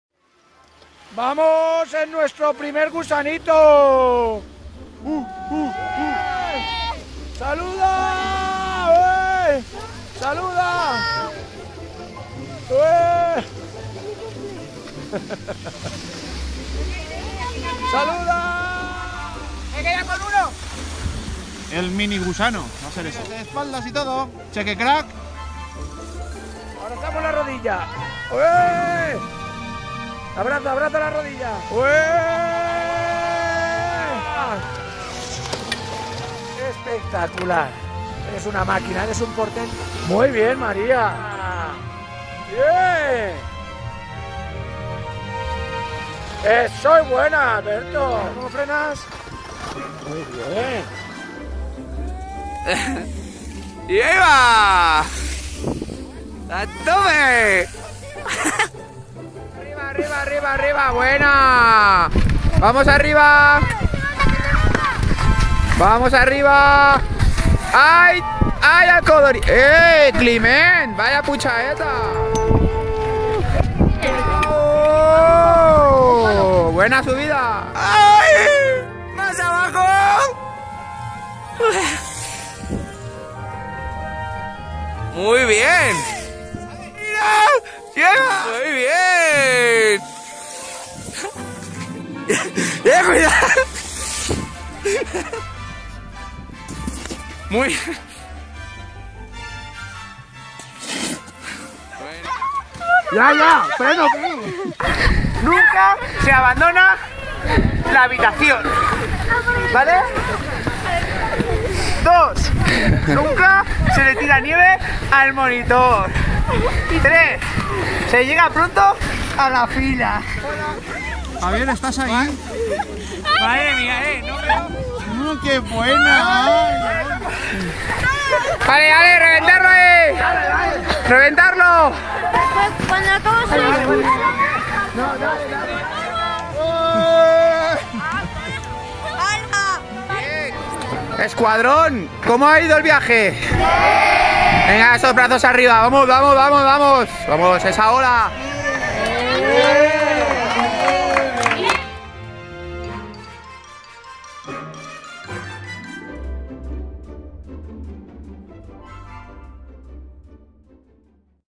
BOÍTAÜLL 2020 (3)
De camino encantados e ilusionados
Mil veces... ¿cuándo llegamos?
6/7 vomiteras😩
Sorteo de habitaciones y traslados de cama si es necesario para que todos estén contentos.
Mucha marcha en la disco y mucha marcha en las habitaciones 😩
Un sol espectacular
Repaso de gafas, guantes, cascos, botas, cremas...😜
Mil veces... ¡no me puedo poner las botas!, ¿me ayudas? 🤪🤪
Muchos nervios
Diversión a tope😆😆
Mil niños felices😁😁😁
Alguna contusión
Caídas de mucha risa
Muchos ¡he subido en telesilla!
Miles ¡qué chulo es esquiar!
Bajadas con los profes haciendo el gusanito🐛
Muchos miedos superados
Una gran fiesta de disfraces
Y profes felices😁😁